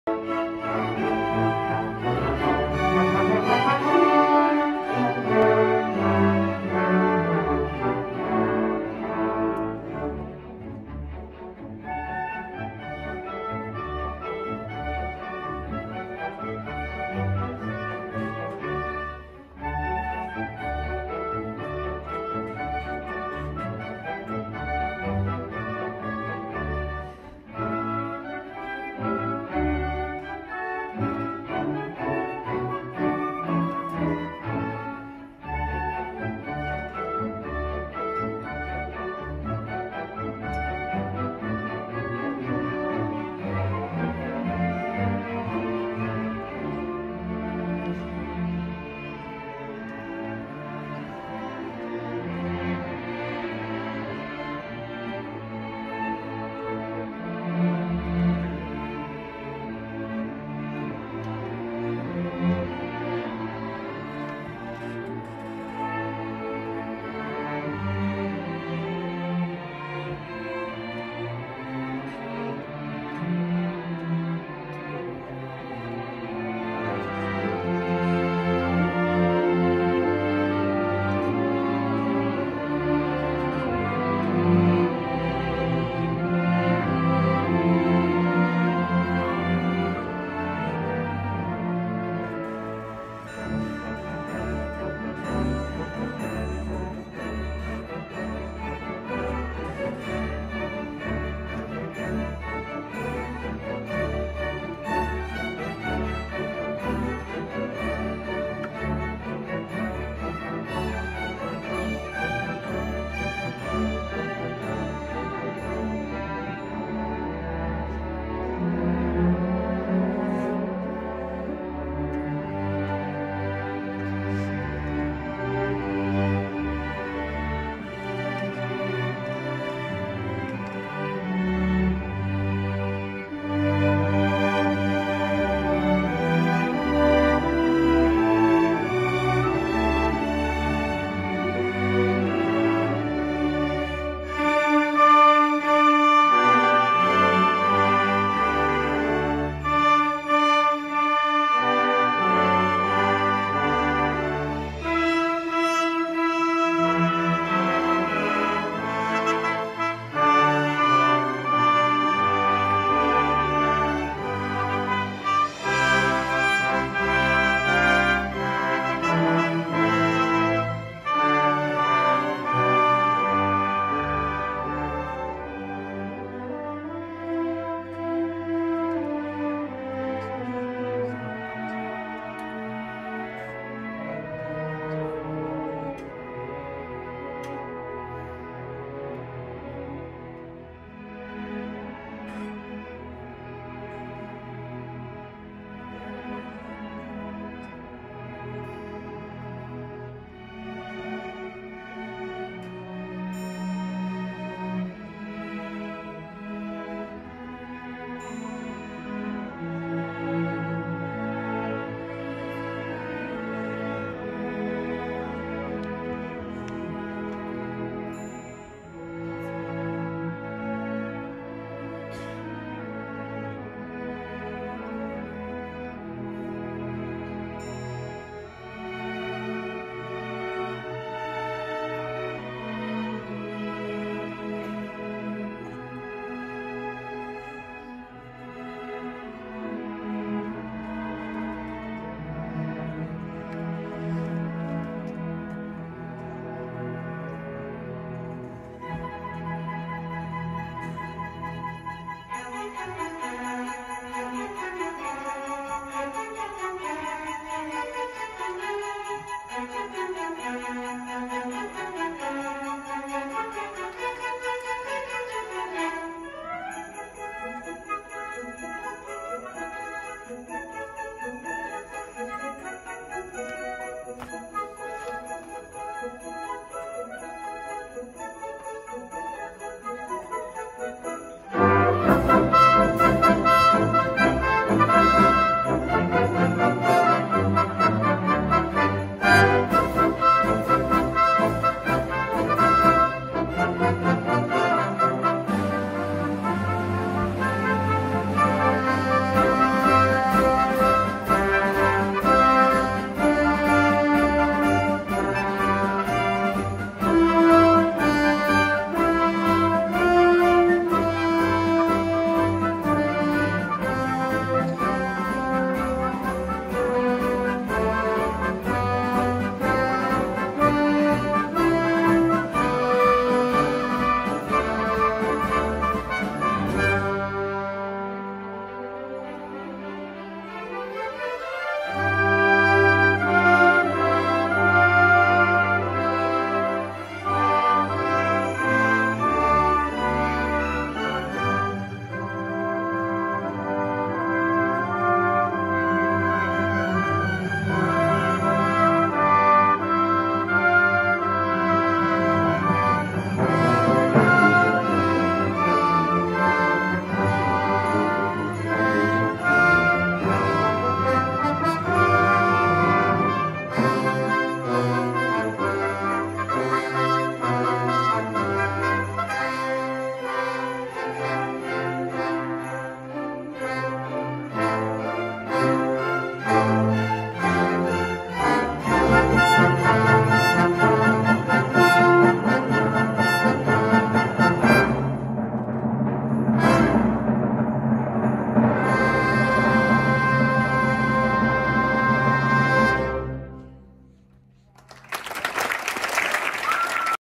Christmas Concert-Denton community orchestra
Christmas Concert-Denton community orchestra with Dallas College Brookhaven Chamber Ensemble